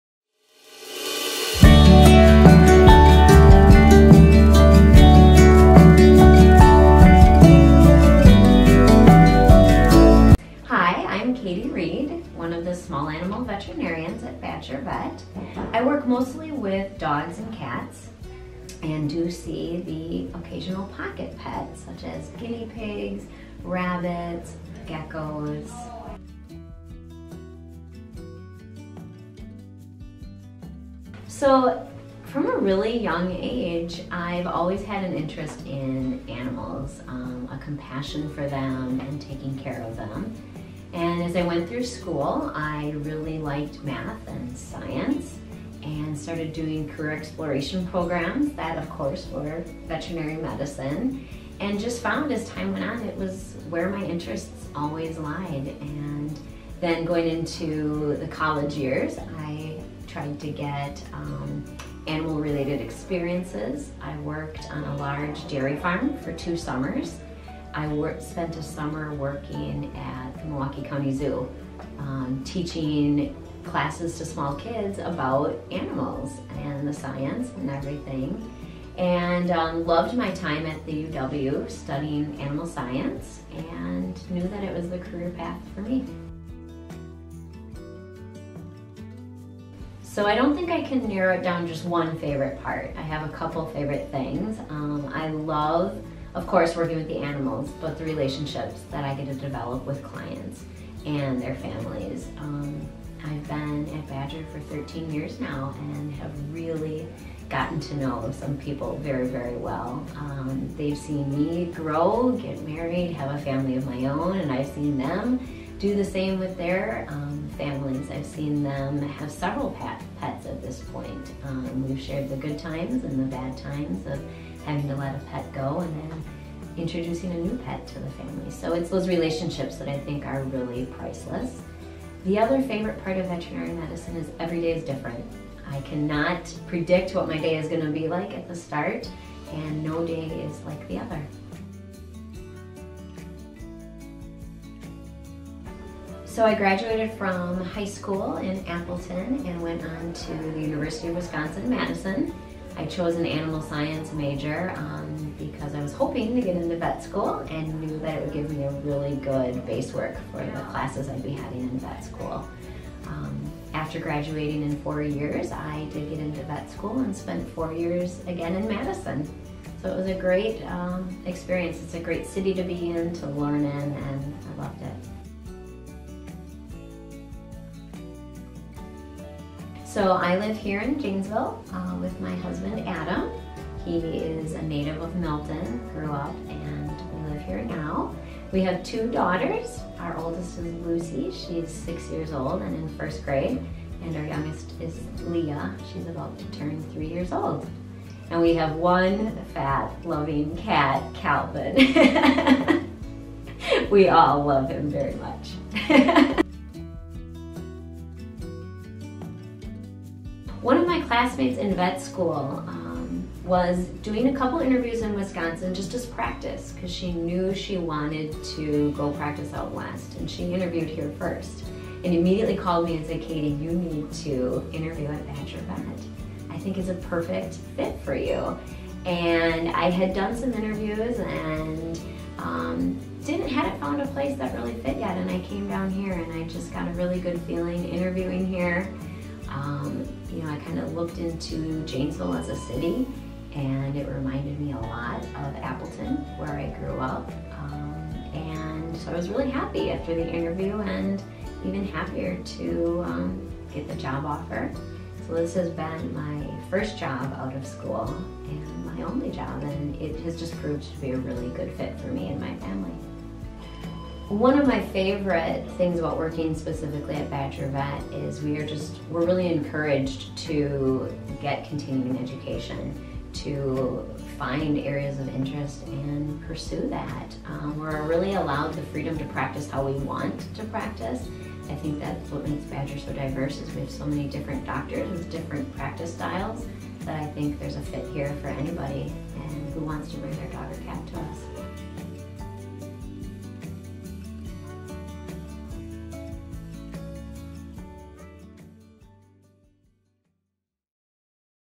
Katie Reed, DVM at Badger Veterinary Hospital
Get to know Katie Reed, DVM, small animal veterinarian at Badger Veterinary Hospital in Janesville, WI.